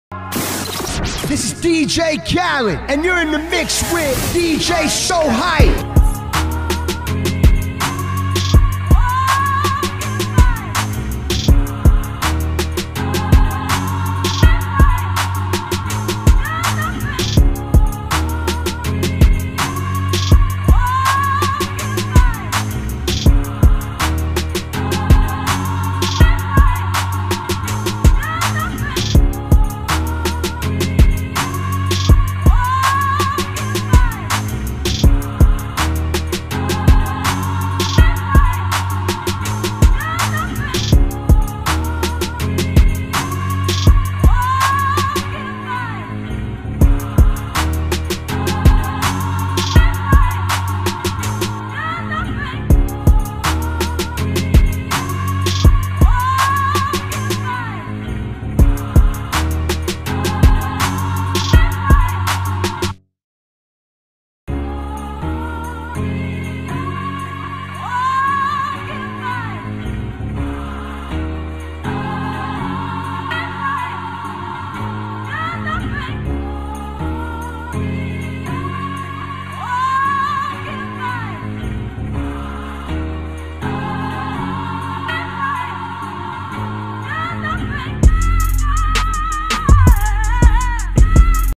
KOBE BRYANT & NIPSEY HUSSLE MURAL PAINTED BY DJ SO HYPE
First time ever doing a mural on a wall and I’m proud to have painted @kobebryant & @nipseyhussle S/o to @matchmyhustle_mmh for allowing me to use the store to do my work. 40 hours in a span of 3 days to finish this piece, I appreciate everybody for the motivation. You can see this mural live at the "Match My Hustle" Store located in Las Vegas on Lake Mead & Jones